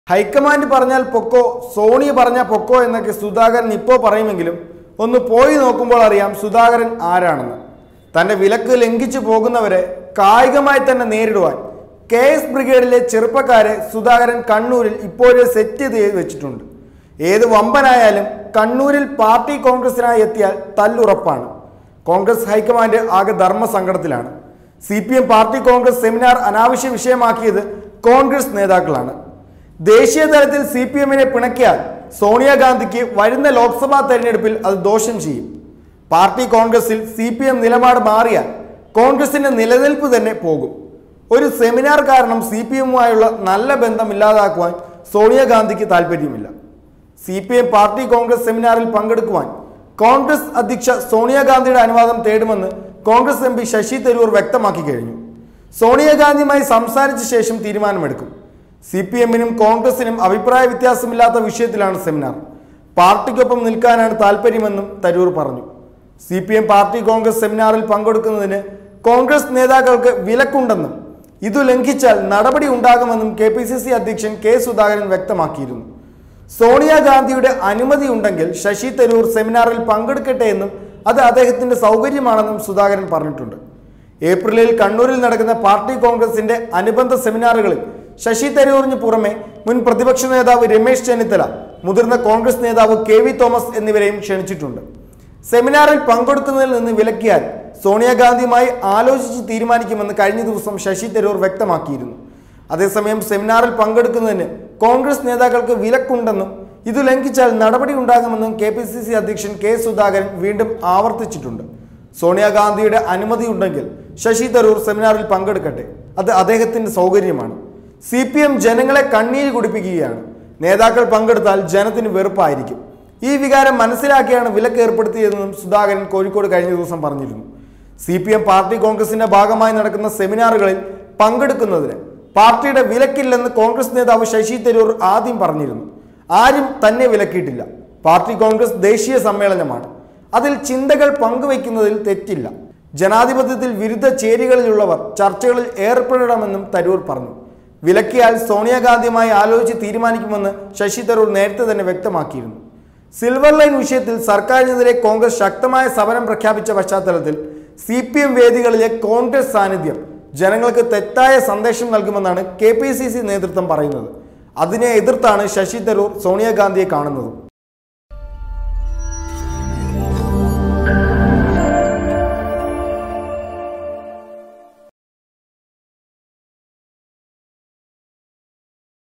സെമിനാറിനായി കണ്ണൂരിൽ കാലുകുത്തുന്ന കോൺഗ്രസ്‌ നേതാക്കളെ തല്ലാൻ ബ്രിഗേഡിനെ ഏർപ്പാടാക്കി സുധാകരൻ
Tag-Generator-Youtube-malayalam news-global